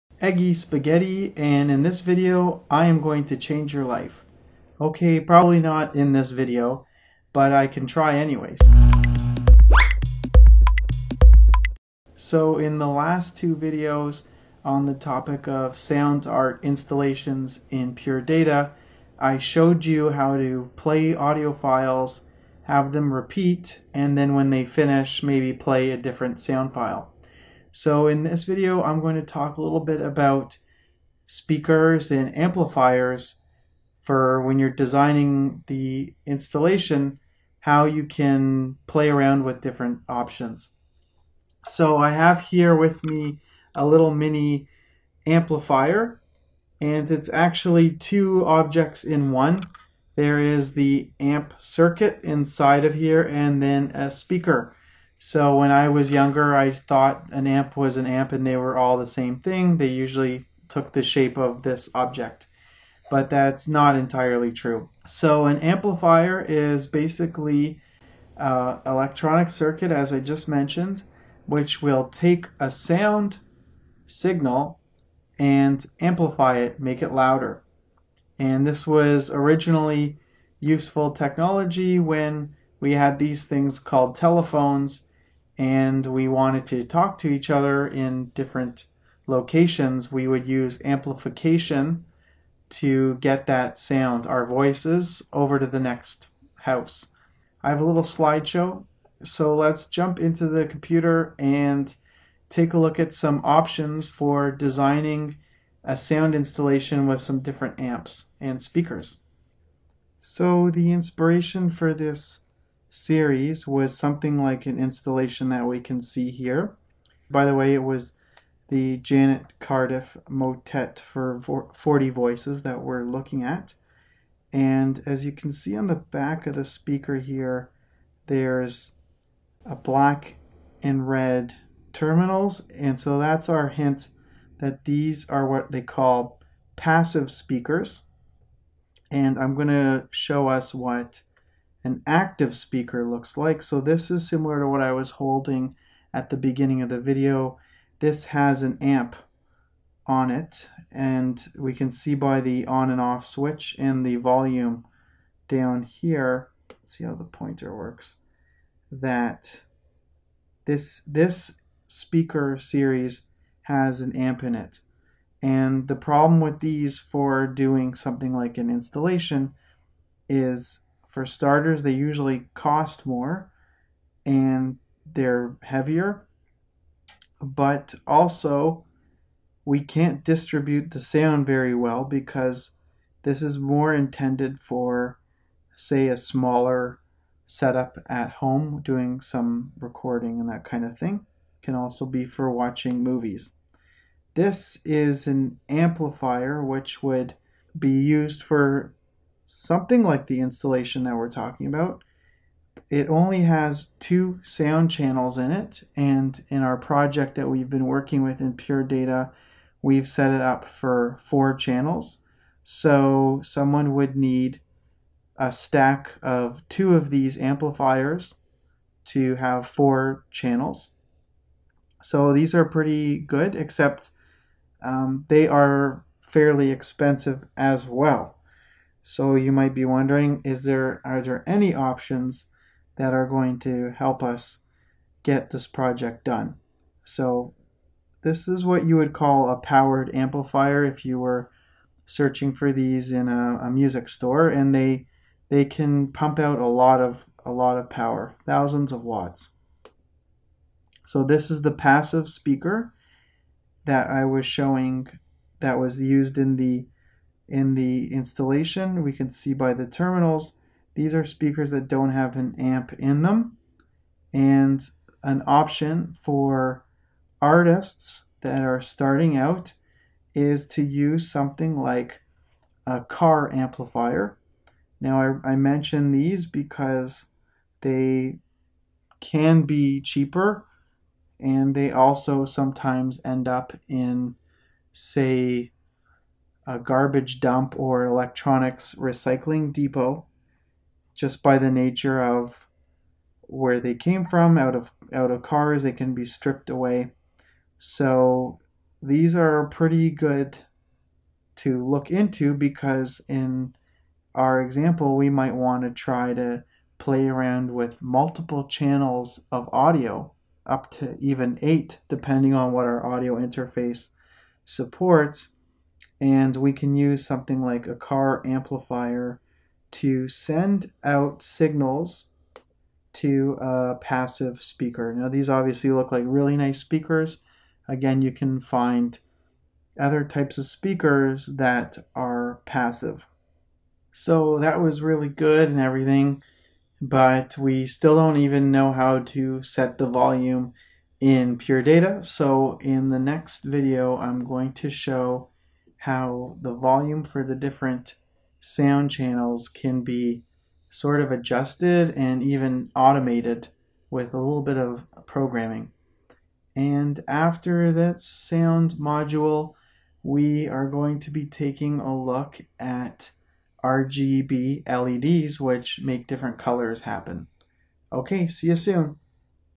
How to Design Sound Art Installations: Amps & Speakers (Part 3)
In this part of our Sound Art Installation series, we take a look at how amps are different than speakers, and some tips for planning a multi-channel project on a budget.